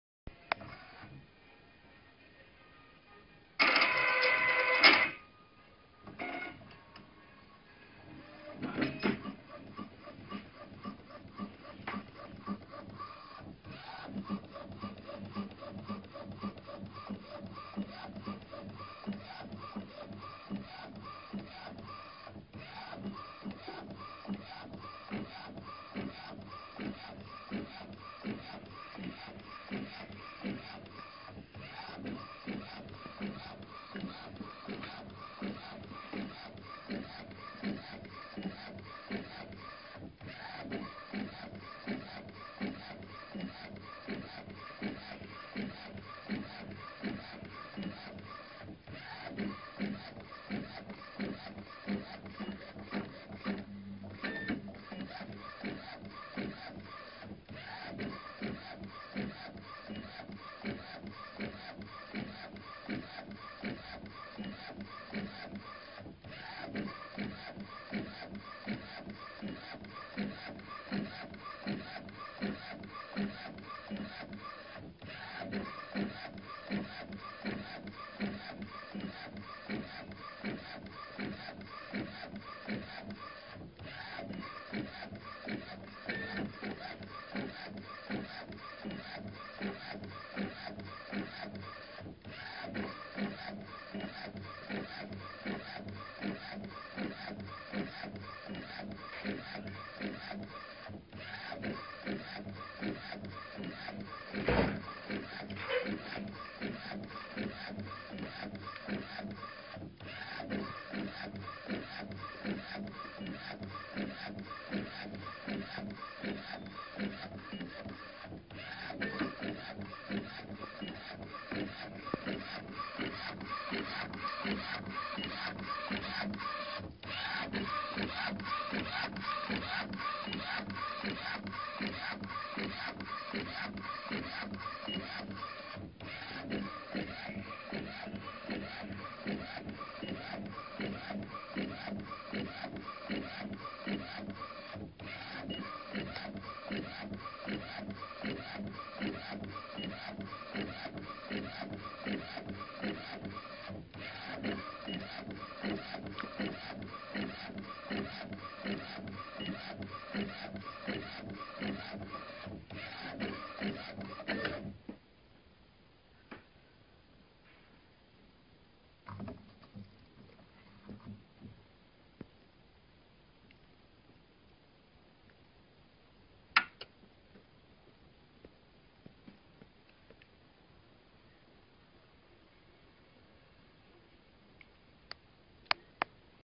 Golf Ball Printing
We can print text, logo's or pictures onto a golfball